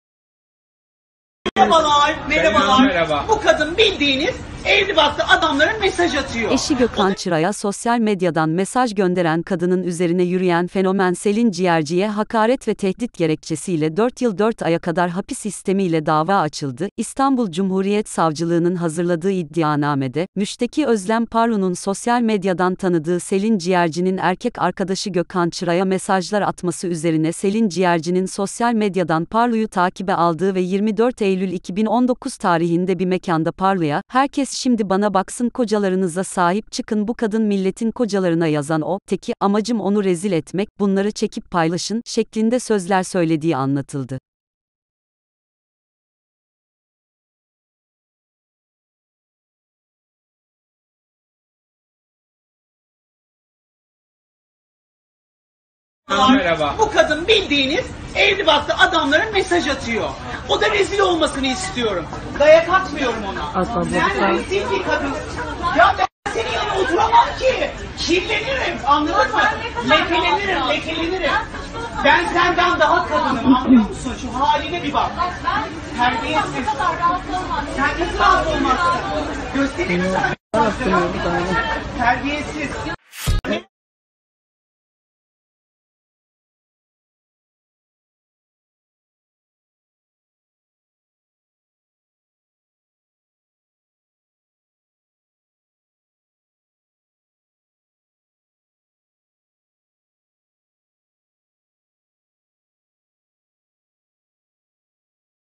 Kocasına mesaj atan kadın Selin Ciğerci'ye hapis istemiyle dava açtı. Selin ciğerci 4 yıl hapis...
Kocasına mesaj atan Özlem Parlu'nun üzerine yürüyen Selin Ciğerci'ye hapis istemiyle dava açıldı
Eşi Gökhan Çıra'ya sosyal medyadan mesaj gönderen kadının üzerine yürüyen fenomen Selin Ciğerci'ye hakaret ve tehdit gerekçesiyle 4 yıl 4 aya kadar hapis istemiyle dava açıldı.
İstanbul Cumhuriyet Savcılığı'nın hazırladığı iddianamede, müşteki Özlem Parlu'nun sosyal medyadan tanıdığı Selin Ciğerci'nin erkek arkadaşı Gökhan Çıra'ya mesajlar atması üzerine Selin Ciğerci'nin sosyal medyadan Parlu'yu takibe aldığı ve 24 Eylül 2019 tarihinde bir mekanda Parlu'ya, "Herkes şimdi bana baksın kocalarınıza sahip çıkın bu kadın milletin kocalarına yazan o… teki, amacım onu rezil etmek, bunları çekip paylaşın" şeklinde sözler söylediği anlatıldı.